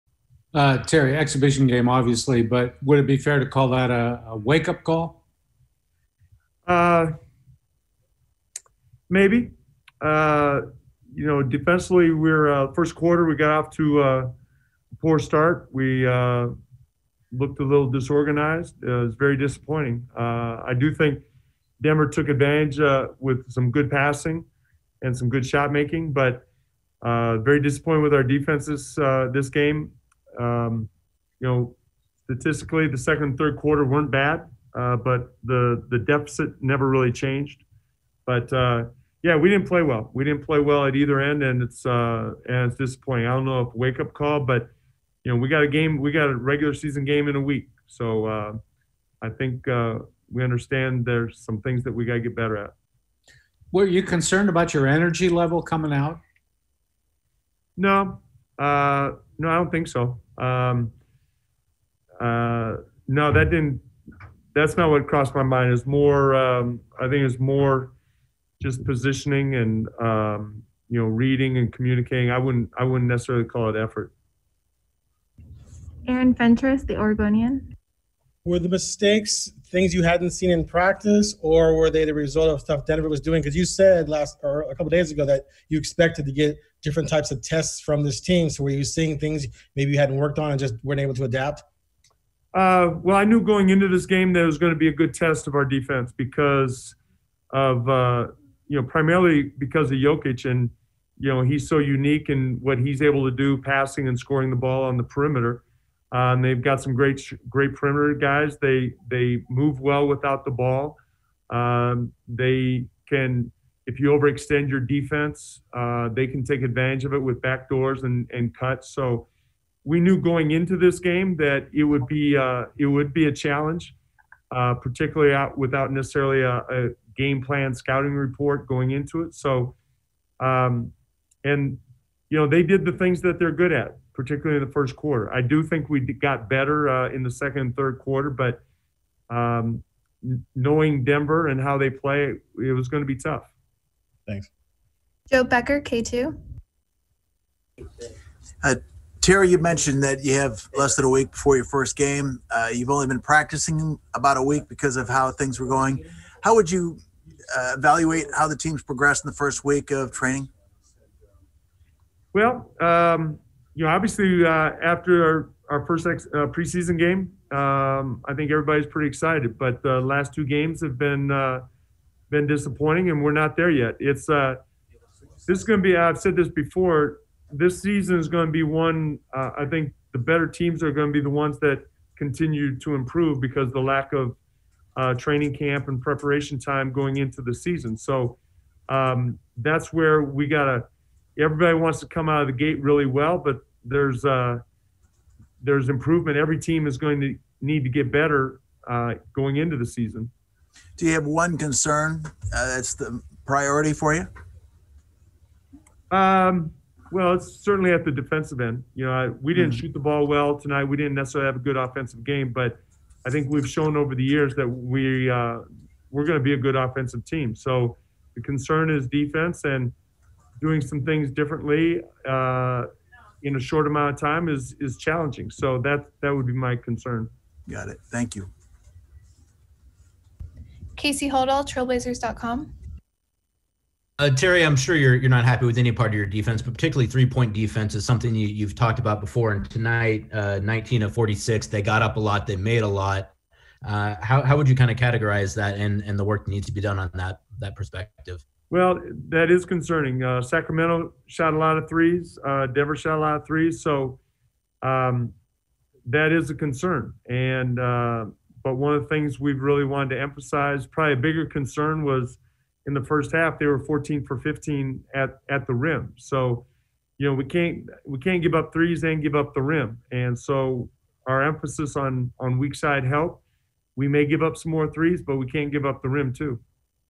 Terry Stotts: "We looked a little disorganized" | Trail Blazers vs. Nuggets
Coach Stotts talks after the preseason loss to Denver on Wednesday.